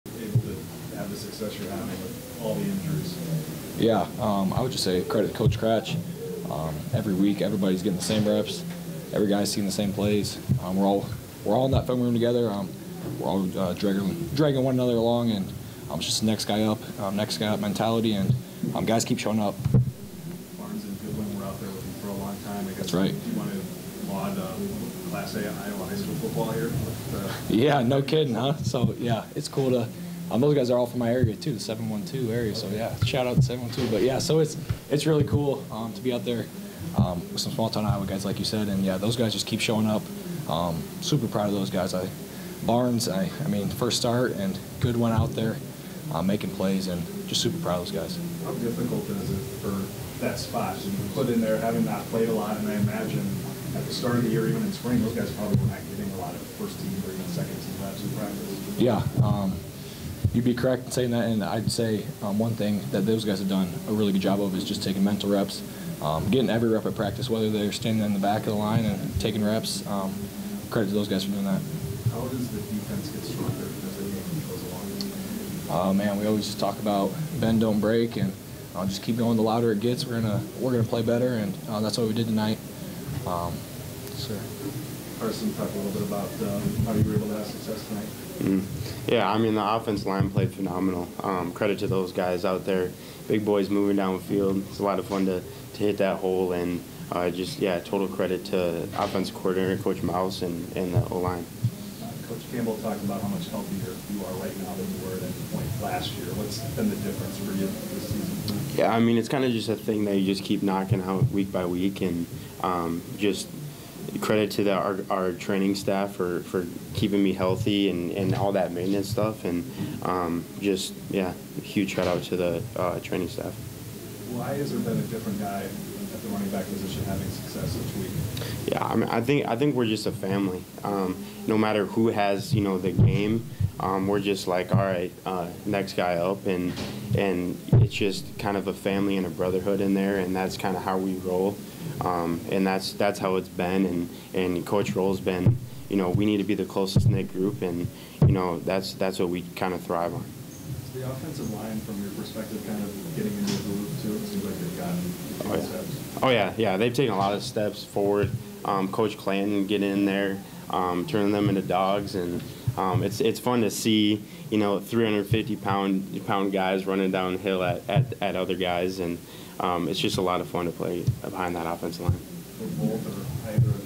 CFTV: Kooper Ebel & Carson Hansen talk win over West Virginia
Iowa State's Kooper Ebel & Carson Hansen spoke after the Cyclones' 28-16 win over West Virginia.